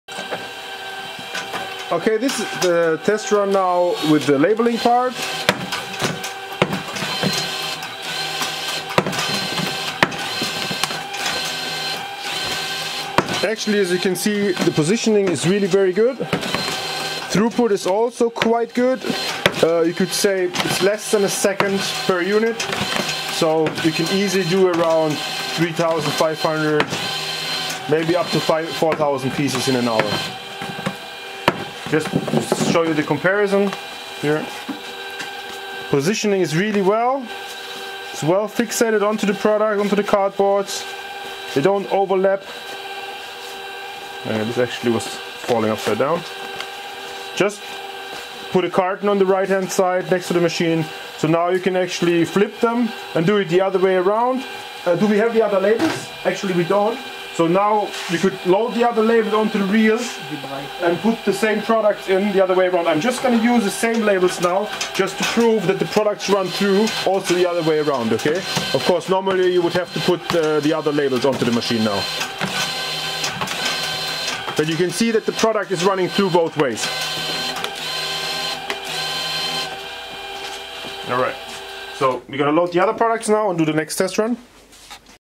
Automatische Etikettierung von Kartonagen auf beiden Seiten mit dem Etikettierer LAB510
Das Video zeigt den Etikettiervorgang von Kartonagen auf beiden Seiten mit dem Etikettierer LAB510, der eine hohe Genauigkeit der Etikettenposition bewirkt.

Mit dem Etikettierer erreichen Sie einen Durchsatz von weniger als einer Sekunde pro Einheit.
Der automatische Etikettierer LAB510 bringt Etiketten auf eine Vielzahl von Produkten auf. Der Etikettierer kann jedes Produkt mit einer flachen Oberfläche bis zu einer maximalen Höhe von 50 mm (2") und einer Breite von 210 mm (8,3") etikettieren.

Setzen Sie sich gerne mit uns in Verbindung, um einen Demotermin zu vereinbaren oder ein Testvideo von Ihren Produkten anzufordern, die von unseren Etikettierern etikettiert werden: